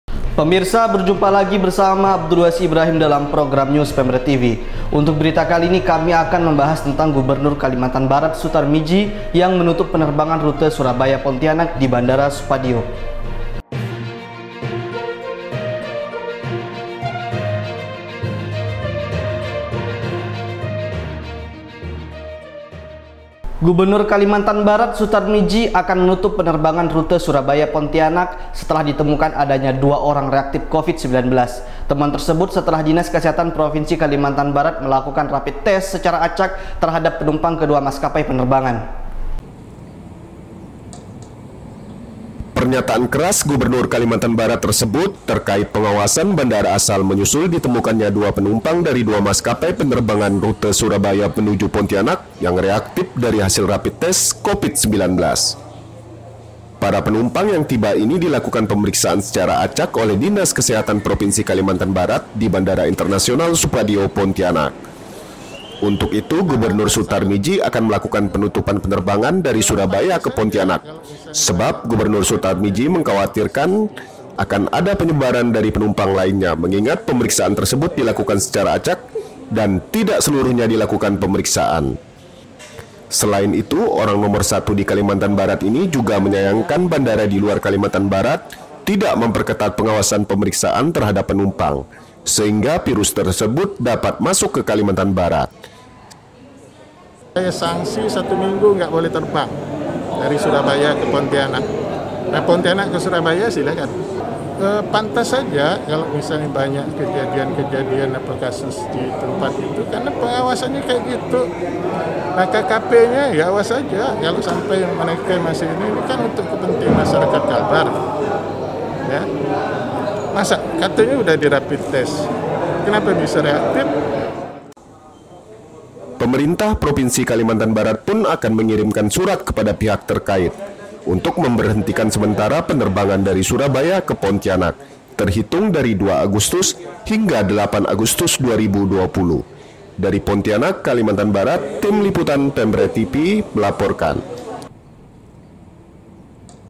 Gubernur Kalbar Larang 2 Maskapai dari Surabaya Terbang ke Pontianak
PONTIANAK, SP - Gubernur  Kalimantan Barat  Sutarmidji  akan menutup penerbangan rute Surabaya – Pontianak, setelah di temukan adanya dua orang reaktif Covid-19. Temuan tersebut setelah Dinas Kesehatan Provinsi Kalimantan Barat  melakukan rapid test secara acak terhadap penumpang ke dua maskapai penerbangan tersebut.

Pernyataan keras Gubernur  Kalimantan Barat tersebut,  terkait pengawasan dari bandara asal, menyusul ditemukannya 2 penumpang dari dua maskapai penerbangan rute Surabaya menuju Pontianak  yang reaktif dari hasil rapid test Covid-19.

Para penumpang yang tiba ini, dilakukan pemeriksaan secara acak oleh Dinas Kesehatan Provinsi Kalimantan Barat  di bandara internasional  Supadio.

Untuk itu, orang nomor satu di Kalimantan Barat ini akan melakukan penutupan rute penerbangan dari Surabaya ke Pontianak,  sebab  Gubernur Sutarmidji mengkhawatirkan akan ada penyebaran dari penumpang lainnya.  Mengingat,  pemeriksaan tersebut dilakukan secara acak dan tidak seluruhnya dilakukan pemeriksaan.

Selain itu,  orang nomor satu di Kalimantan Barat ini,  juga menyayangkan bandara di luar Kalimantan Barat  tidak memperketat pengawasan pemeriksaan terhadap penumpang, sehingga virus tersebut dapat masuk ke Kalimantan Barat.

Pemerintah Provinsi Kalimantan Barat pun akan mengirimkan surat kepada pihak terkait, untuk memberhentikan sementara penerbangan dari Surabaya ke Pontianak, terhitung dari 2 Agustus hingga 8 Agustus 2020.

Saksikan berita selengkapnya, dalam tayangan News PemredTV diatas.